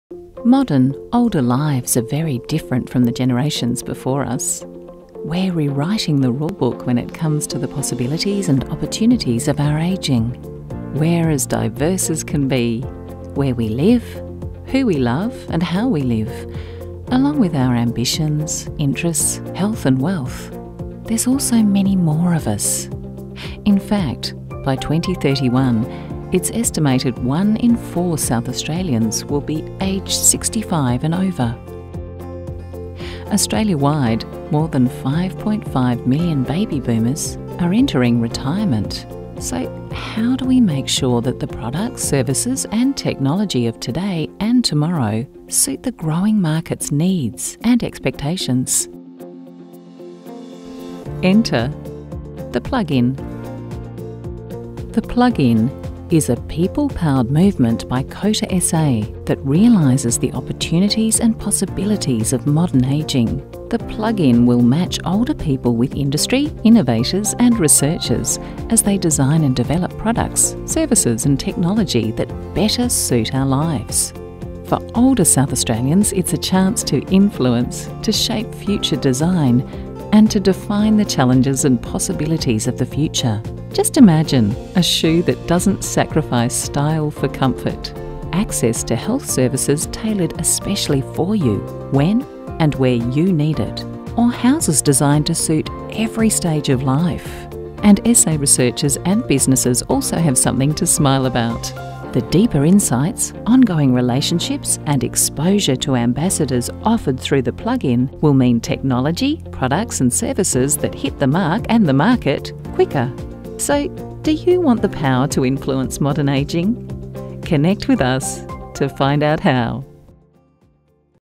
Do you want the power to influence the future of ageing?
We invite you to join us and have the opportunity to work alongside industry, innovators and researchers to help create technology, products and services that will better meet the future needs of older people.